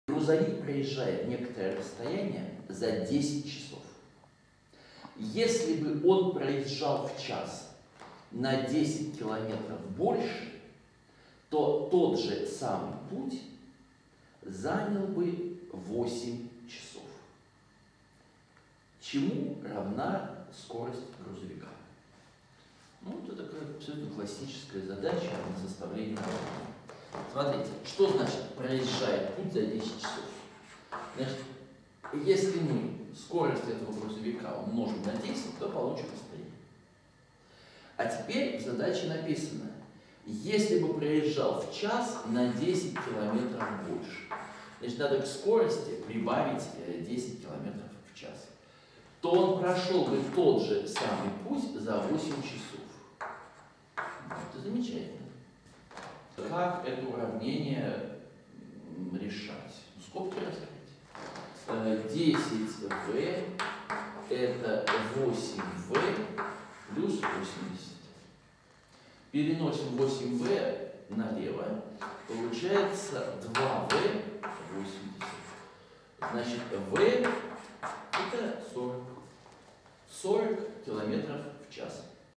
393. Грузовик проезжает некоторое расстояние за 10 часов
Грузовик проезжает некоторое расстояние за 10 часов. Если бы он проезжал в час на 10 километров больше, то тот же путь занял бы 8 часов. Чему равна скорость грузовика?
Задача 393 «Тысячи и одной задачи по математике» А.В. Спивака.